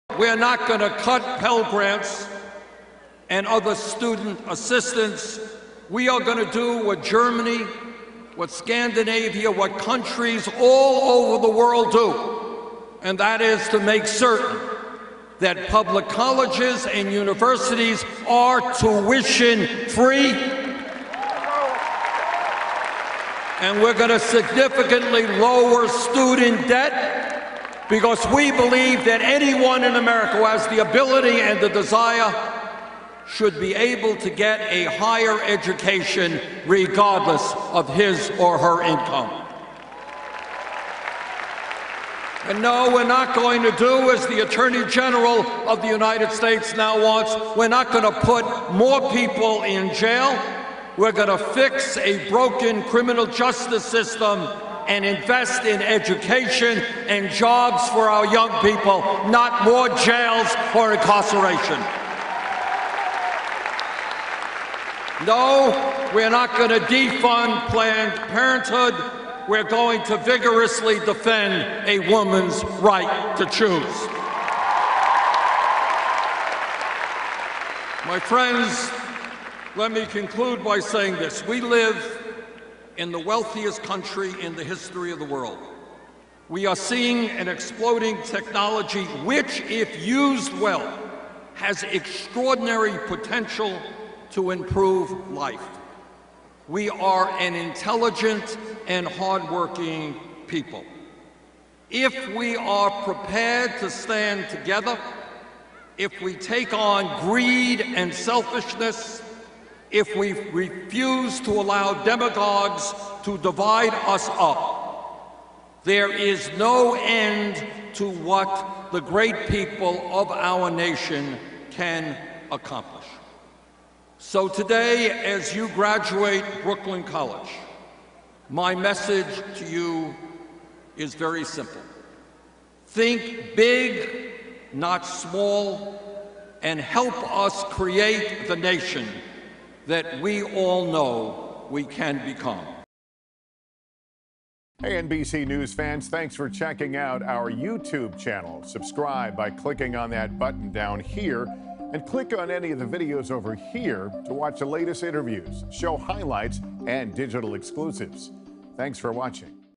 Bernie Sanders To Brooklyn College Graduates: 'Think Big' | NBC News
Bernie Sanders give the commencement address at Brooklyn College, telling graduates to 'think big.'